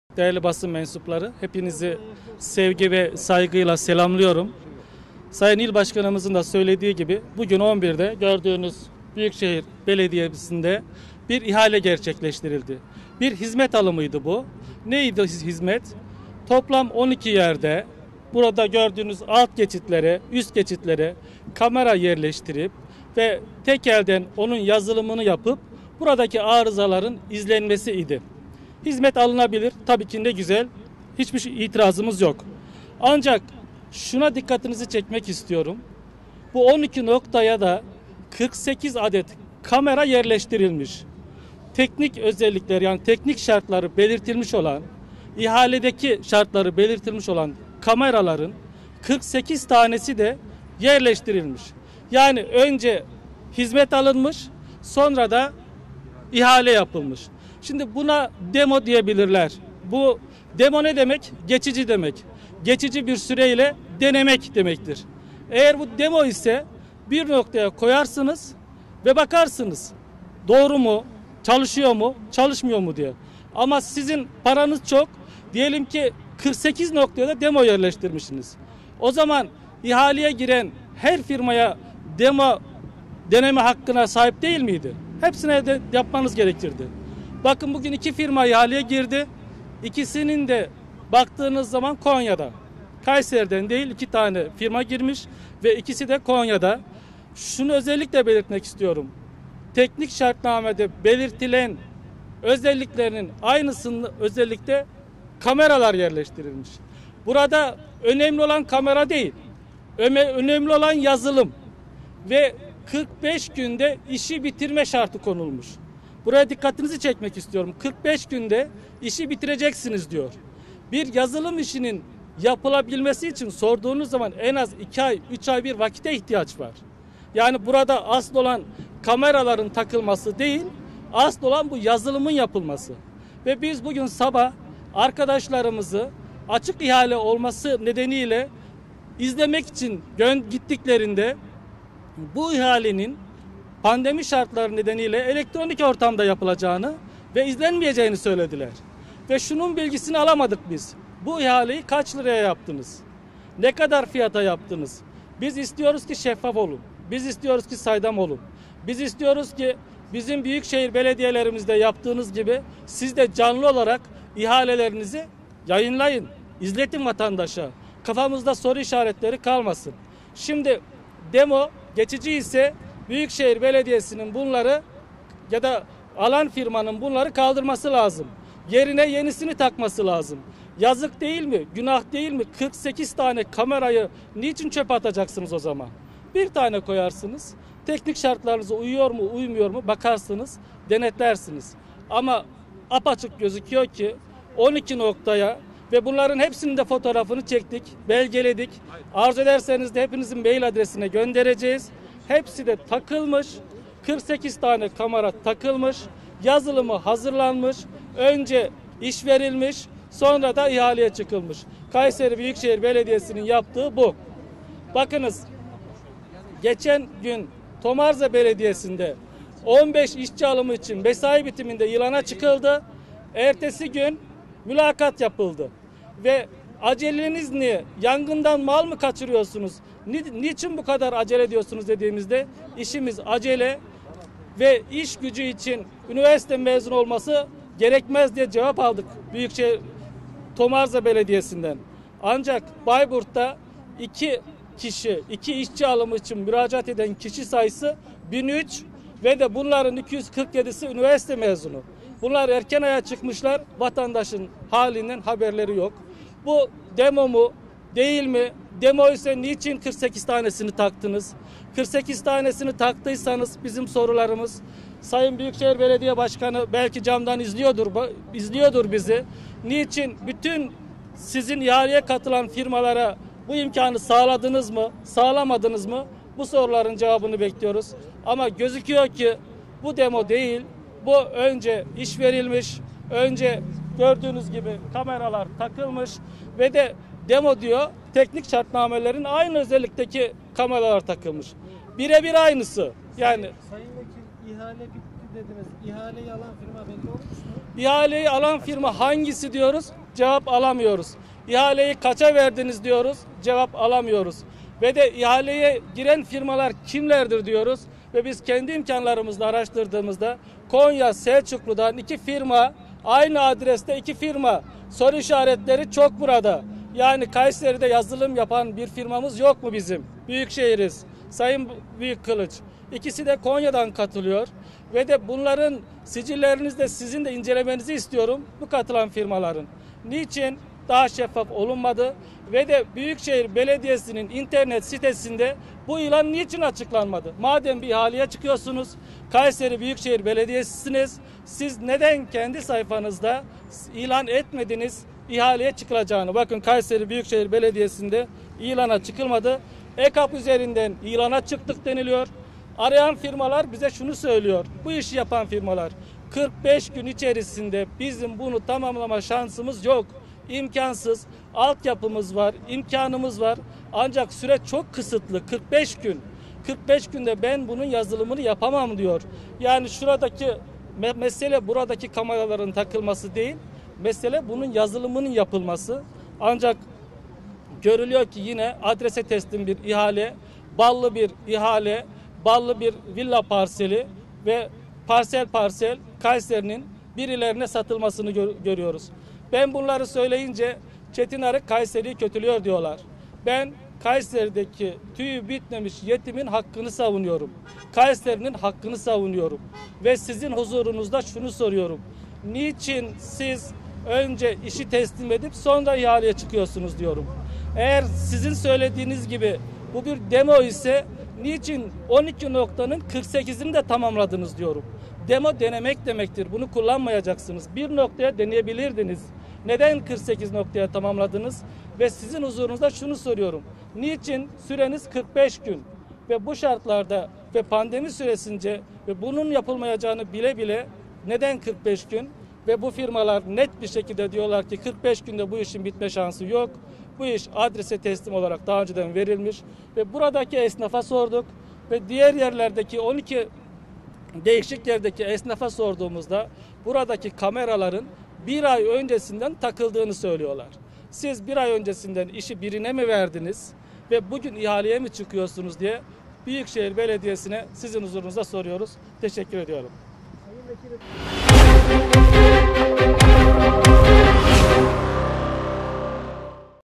ARIK’TAN ÇARPICI İDDİA: “ÖNCE İŞİ YAPTIRDILAR SONRA İHALE…”
Kayseri Büyükşehir Belediyesi'nde bügün saat 11'de gerçekleştirilen ihalenin ardından CHP Kayseri Milletvekili Çetin Arık Büyükşehir Belediyesi önündeki alt geçit önünde basın açıklaması yaptı.

Cumhuriyet Halk Partisi Kayseri Milletvekili Çetin Arık, Kayseri Büyükşehir Belediyesi’nin yaya alt ve üst geçitlerinde bulunan özürlü asansörlerin, yürüyen merdivenlerin arızalarının uzaktan tespiti ve 7/24 izlenmesi için uzaktan kontrol sistemi kurulması işinin önce yaptırıldığını sonra ihalesinin yapıldığını ifade etti.

"KAMERALAR BİR AY ÖNCE TAKILDI"

Kayseri Büyükşehir Belediyesi’nin 19 Mart 2021 tarihinde saat 11.00’de Kayseri Büyükşehir Belediyesi Destek Hizmetleri Daire Başkanlığı tarafından; Kayseri Büyükşehir Belediyesine Ait Yaya Alt ve Üst Geçitlerinde Bulunan Özürlü Asansörlerin, Yürüyen Merdivenlerin Arızalarının Uzaktan Tespiti ve 7/24 İzlenmesi İçin Uzaktan Kontrol Sistemi Kurulması işi için ihale yaptığını belirten Arık, ihale sonucunun henüz belli olmamasına rağmen, ihalede konu edilen kameraların yaklaşık bir ay öncesinden takıldığını söyledi. 12 noktaya 48 Adet kamera takıldığını ifade eden Arık, kameranın olayın görünen kısmı olduğunu, işin asıl büyüklüğünün yazılım olduğunu söyledi.

"KAYSERİ'DE HİÇ Mİ YAZILIM FİRMASI YOK?"

Takılan kameralar ile şartnamede istenen kameraların birebir aynı olduğunu ifade eden Arık, “Görülen o ki, önce işi yaptırmışlar şimdi de ihalesini yapıyorlar. Bize ulaşan firmalar 45 günde bu yazılımın olamayacağı ve işin zaten birilerine yaptırıldığını belirterek, kendilerinin ihaleye girmediklerini söylediler. Belediyeden yetkililer bu kameraların demo olduğunu söylüyor. Demo dediğiniz şey, deneme amaçlı bir iki yere takılır. Şartnamede yer alan tüm noktalara kameralar takılmış. Ayrıca ihaleye giren firmalar açısından tüm firmalara demo takmaları için bir çağrıları oldu mu, hiç sanmıyorum. Yaptığımız incelemede ihaleye Konya Şelçuk’tan iki firmanın katıldığını tespit ettik. Kayseri’de hiç mi yazılım firması yok?” diye sordu.

Mimarsinan Yaya Alt geçidi, Yeraltı çarşısı camii girişi, yeraltı çarsısı vilayet girişi, yeraltı çarsısı medrese girişi, yeraltı çarşısı kurşunlu girişi, orduevi yaya alt geçidi, Melikgazi yaya alt geçidi, Kocasinan Yaya alt geçidi, Büyükşehir Belediyesi Tramvay Durağı Alt geçidi, Fuzuli Tramvay Durağı, Millet Caddesi Bariyer Girişi, Mille Caddesi Bariyer Çıkışı olmak üzere 12 noktaya toplam 48 adet kamera takıldığını belirten Arık, tüm noktaları tek tek incelediklerini ve kameraların bir ay kadar önce takıldığını belirtti.

Arık, Büyükşehir Belediyesi’nin söz konusu ilanı kendi sayfasından duyurmamasının, sadece EKAP üzerinden ilana çıkmasının da düşündürücü olduğunu sözlerine ekledi.